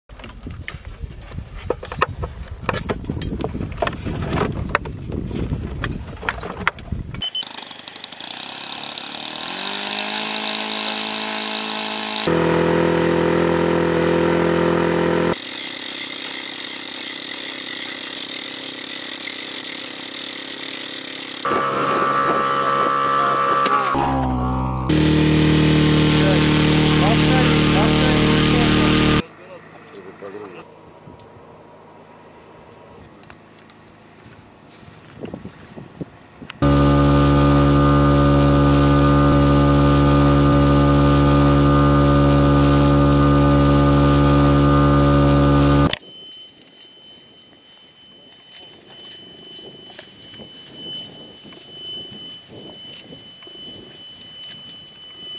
Мониторинг лесных пожаров расчетами БпЛА в Сибири
Расчеты комплексов беспилотных летательных аппаратов «Орлан-10» Центрального военного округа приступили к мониторингу очагов возгорания в лесистой местности вблизи военных объектов на территории Кемеровской области и Алтайского края.

Воздушная разведка ведется на высоте от 300 до 1000 метров и на удалении от пункта управления БпЛА «Орлан-10» до 100 км.